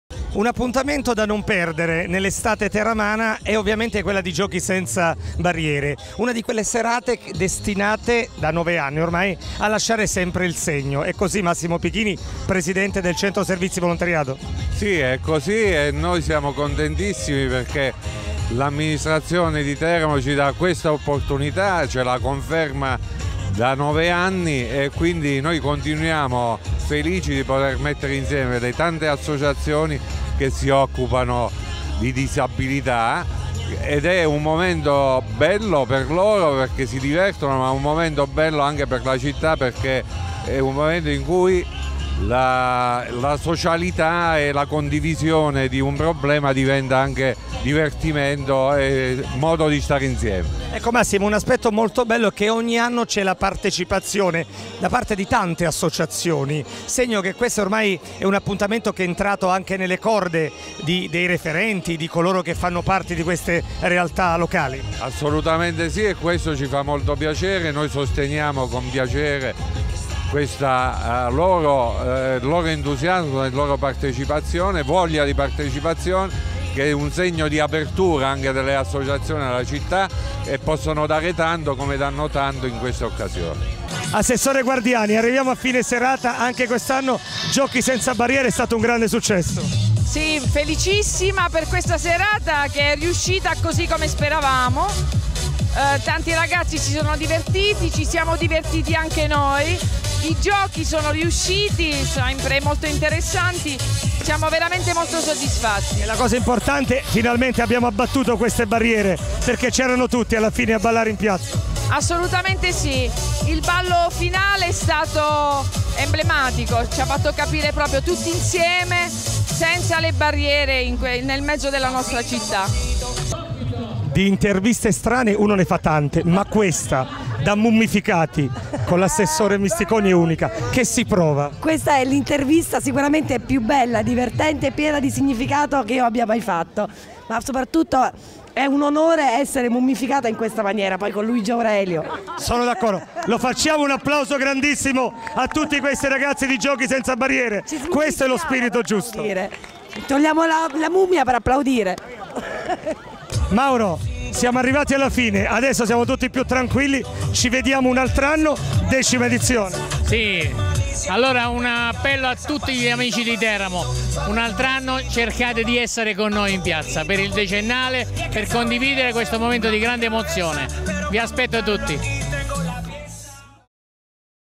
TERAMO, FOLLA ED EMOZIONI DURANTE “GIOCHI SENZA BARRIERE” ORGANIZZATO DAL CSV
Servizio di J News - 15 luglio 2017